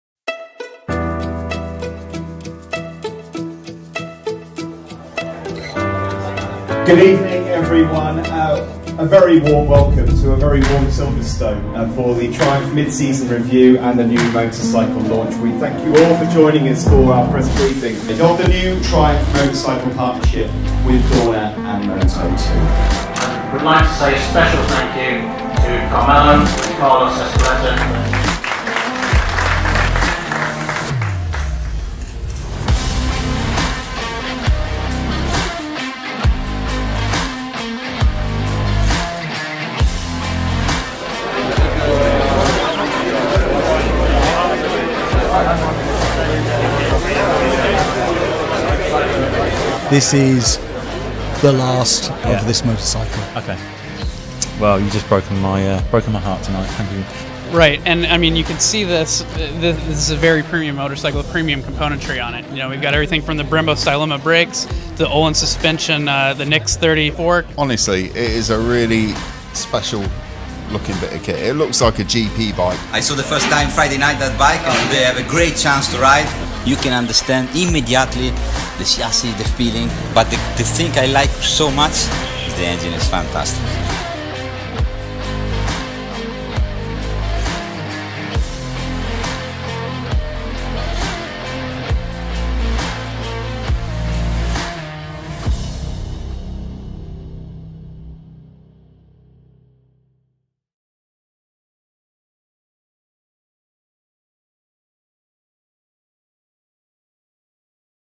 💥 The New Triumph Daytona 765 Moto2 - Unveiling
Watch the official video of the unveiling of the new Triumph Daytona 765 Moto2 motorcycle